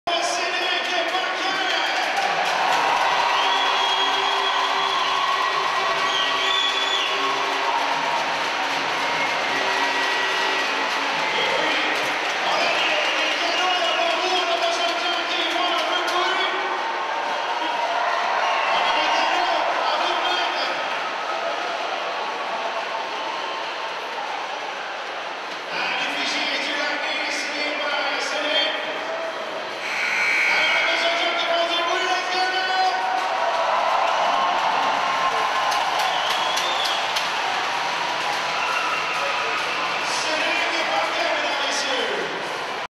Armada 150123 Cedric Paquette
L'attaquant du Lightning de Tampa Bay, Cédric Paquette, rend visite à son ancienne équipe junior, L'Armada de Blainville-Boisbriand, lors de la pause du match des étoiles de la lnh